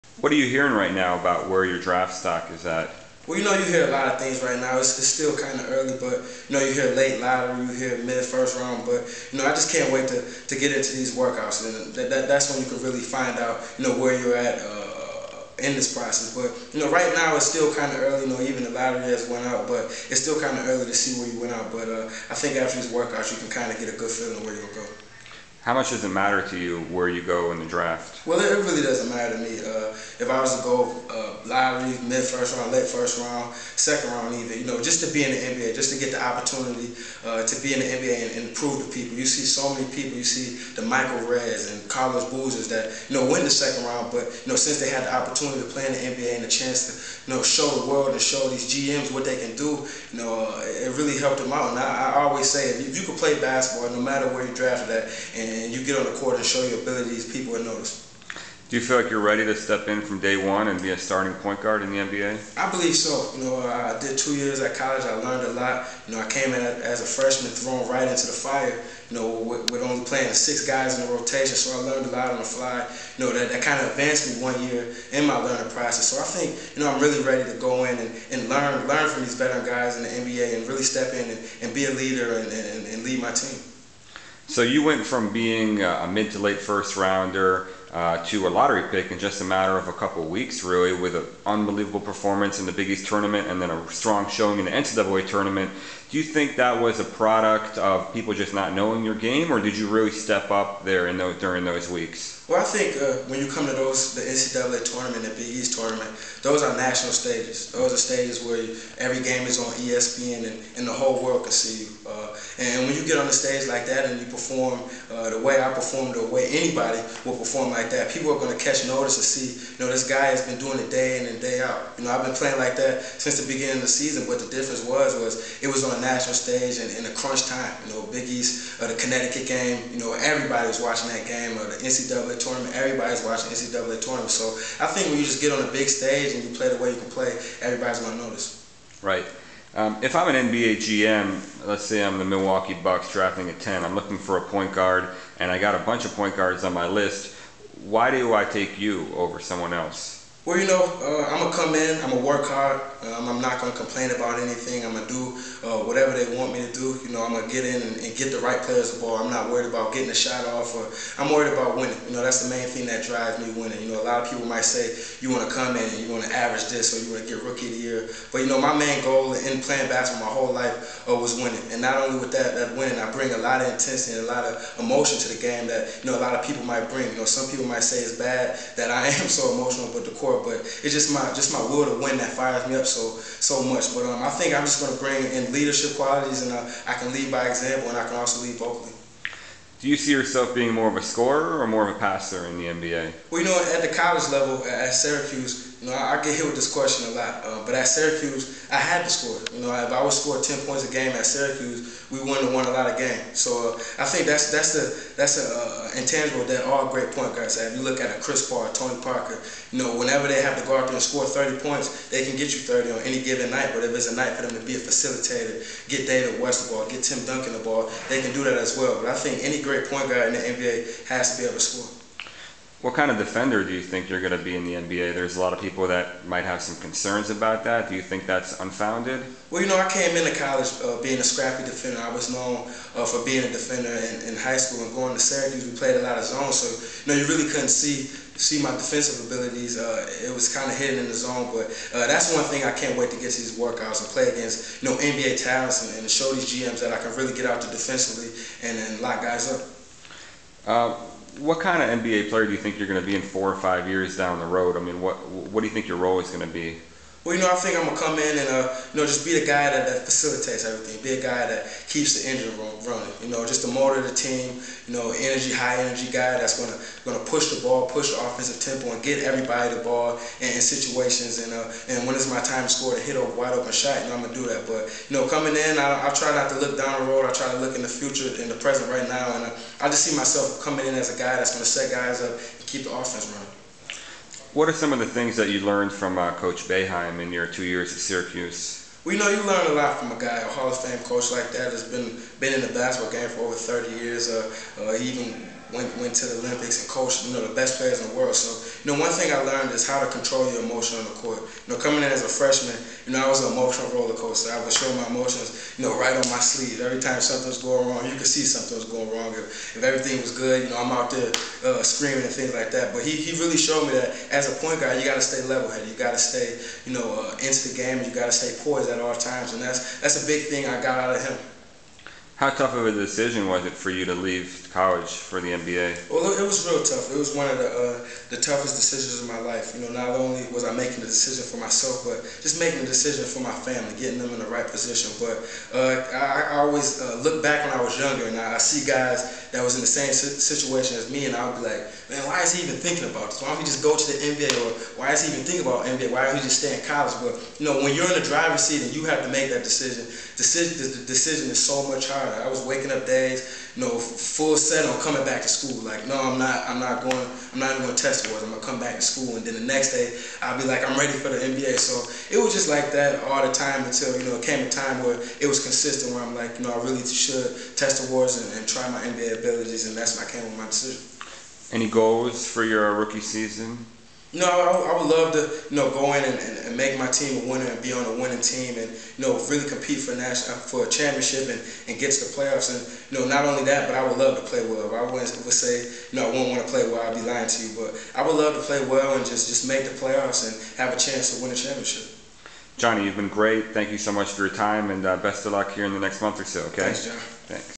Jonny Flynn interview with DraftExpress.com, Part 2
(Part Two) Jonny Flynn sits down for a chat with Jonathan Givony of DraftExpress.colm at Tim Grover's A.T.T.A.C.K Athletics facility in Chicago.